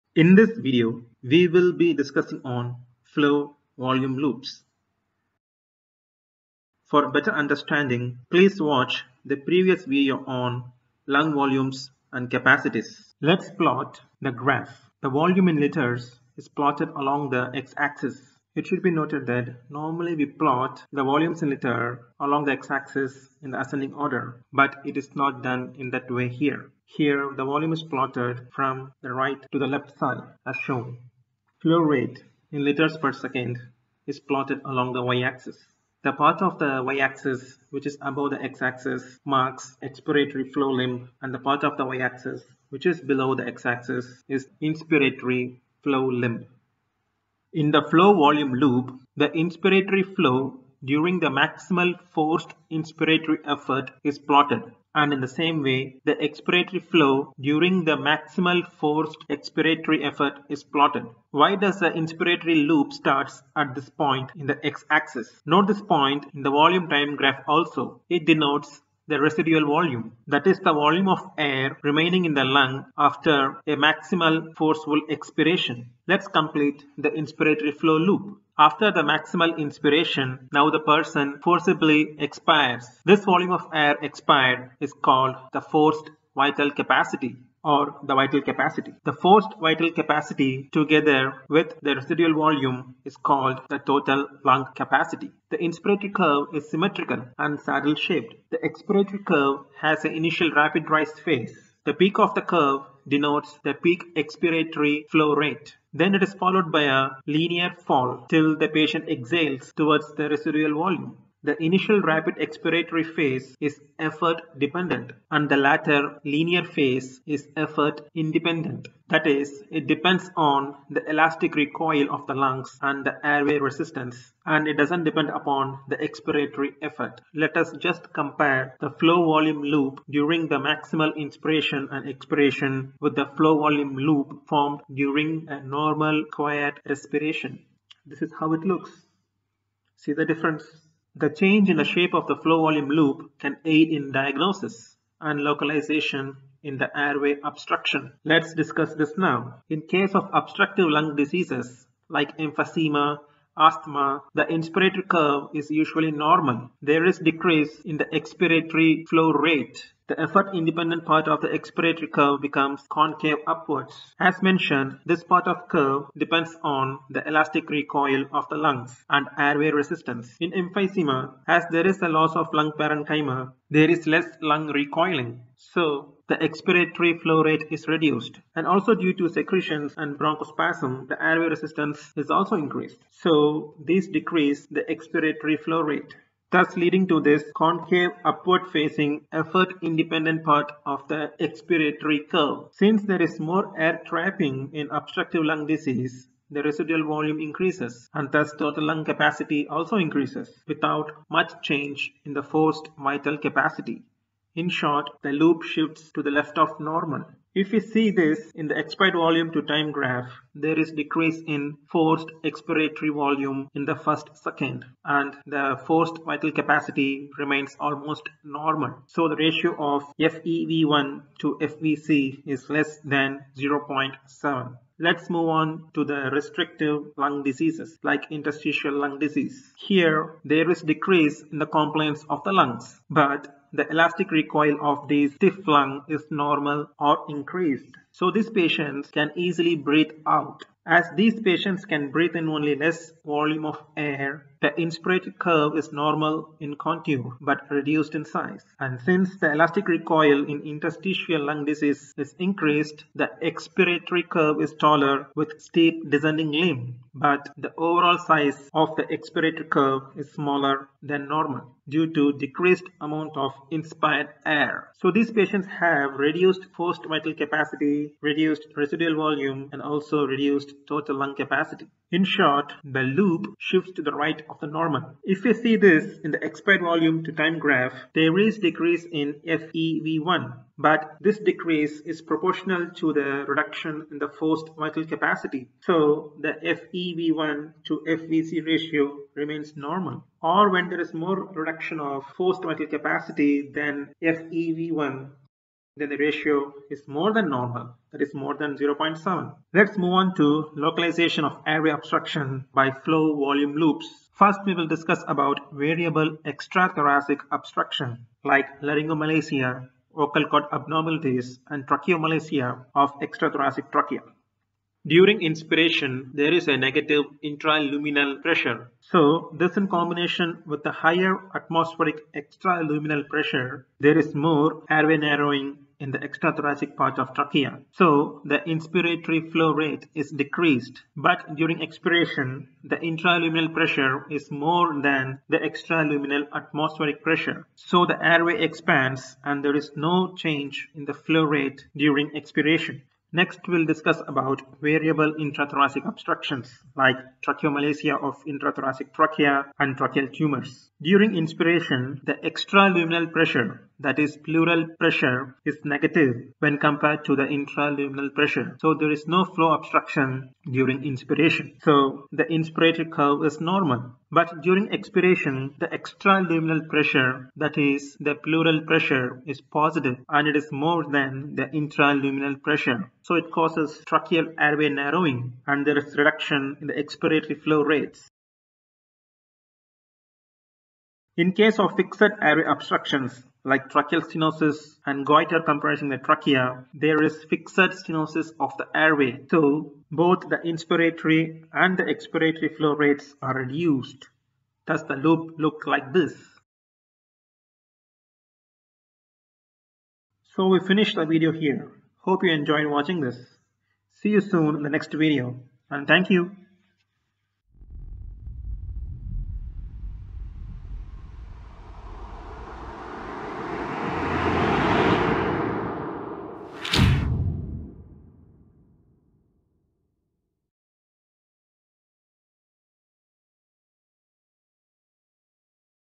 Flow Volume Loop | PFT | Respiratory Physiology | Made Easy
In this video Flow_volume_loops are discussed in detail.
Before proceeding please watch the video on Lung volumes and capacities...

The topics covered are...
1. Normal flow volume loop
2. Flow volume loop in obstructive lung diseases
3. Flow volume loop in restrictive lung diseases
4. Flow volume loops in variable extrathoracic airway obstruction
5. Flow volume loops in variable intrathoracic airway obstruction
6. Flow volume loops in fixed airway obstruction